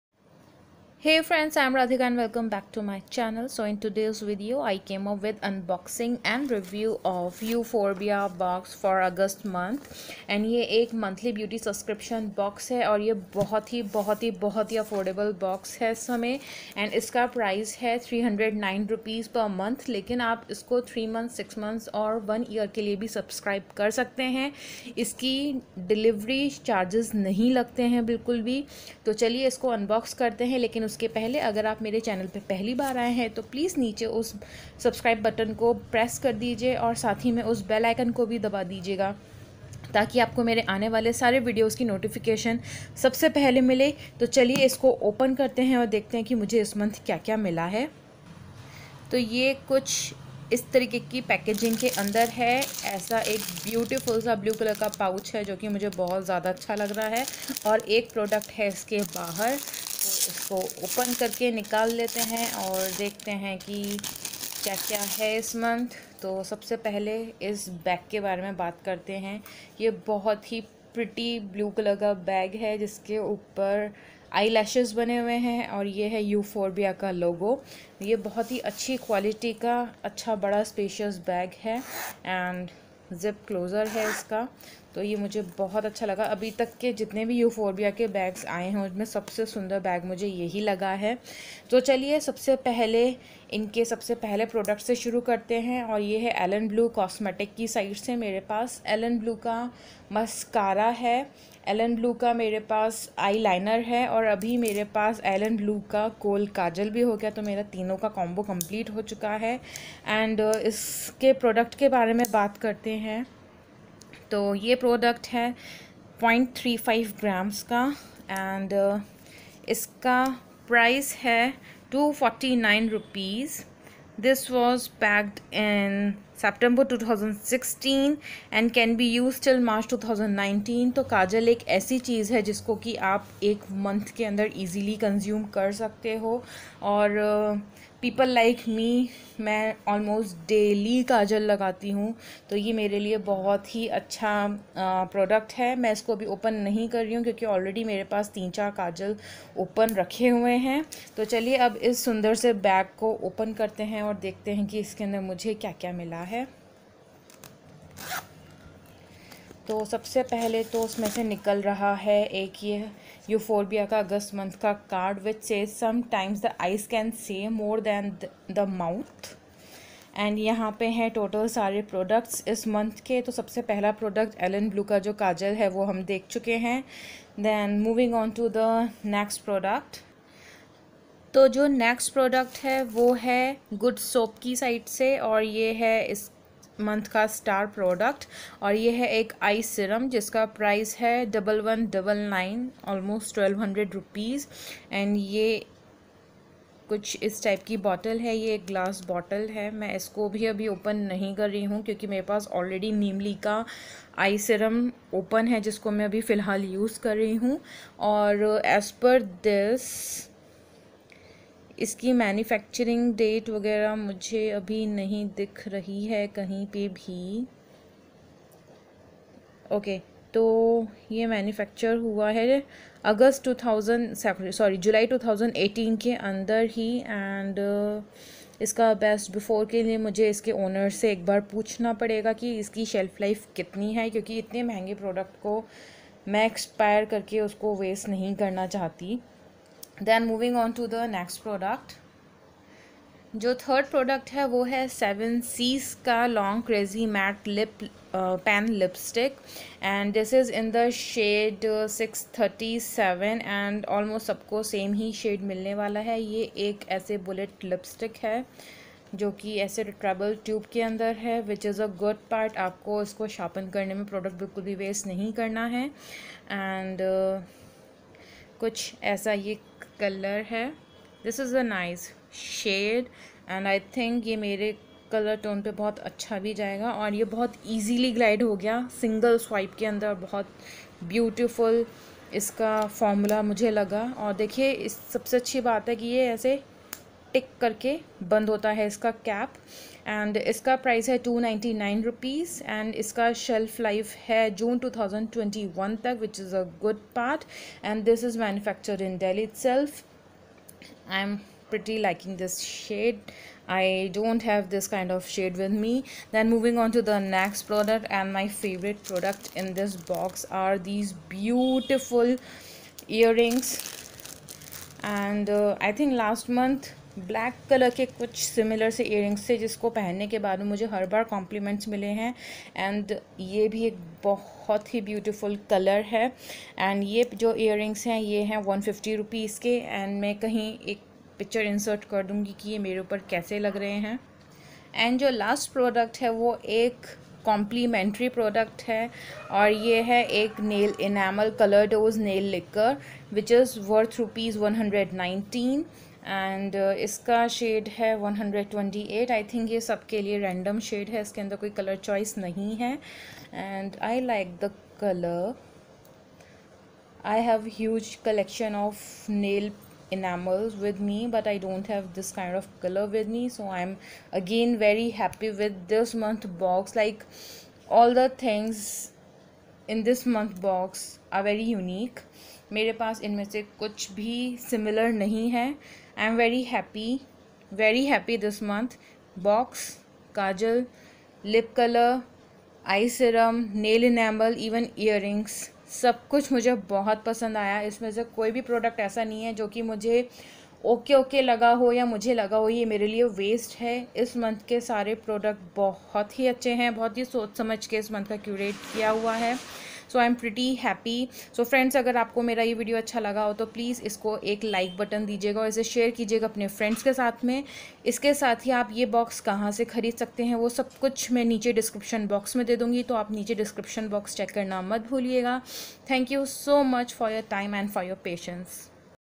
Euphorbia Box | August 2018 | Rs 2000+ products
Hi,

Today's video is an unboxing & review of Euphorbia Box - August 2018 . Euphorbia box one of the most affordable subscription box with a good mix of quality skincare, makeup and Jewellery.

Products in August Euphorbia box:

1. Elenblu Power Khol Kajal (Rs.249)

2. Good Soap Eye Serum(Rs.1199)

3. Seven Seas Long Crazy Matte Pen Lipstick (Rs. 299)

4. A pair of earrings (Rs.150)

5. Color Dose Nail paint ( Complimentary ) Rs. 119

Total worth of August Box Rs. 2000+

Subscription:

12 Months: 259 Rs/ Month (Shipping included)

6 Months: 289 Rs/ Month (Shipping included)

3 Months: 299 Rs/ Month (Shipping included)

1 Month: 309 Rs/ Month (Shipping included)

I love the beautiful comments that all you guys post and I really value and appreciate the time that you take out to watch my videos 😊

Love
Radhika